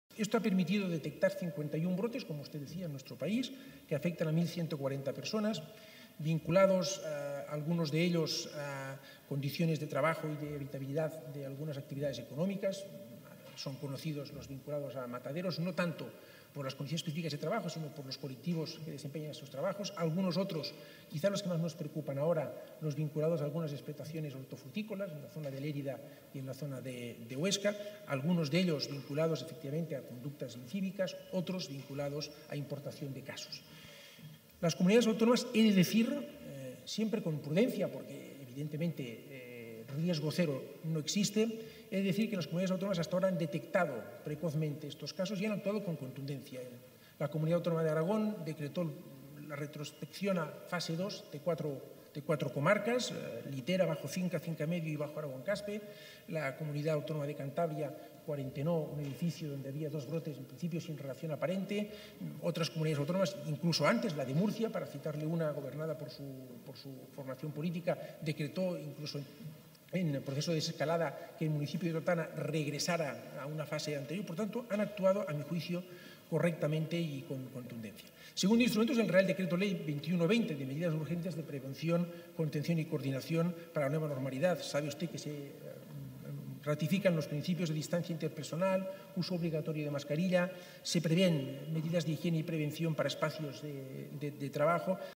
Más de 1.100 personas se han infectado del Covid-19 en los 51 brotes
Más de 1.100 personas se han infectado del Covid-19 en los 51 brotes ocurridos en España, según ha informado el ministro de Sanidad, Salvador Illa, en una respuesta a una interpelación en el Senado formulada por el senador del Grupo Parlamentario Popular, Antonio Román Jasanada, quien le ha pedido que informe sobre las medidas previstas para la prevención y control de los posibles rebrotes en España.

(Fuente: Senado)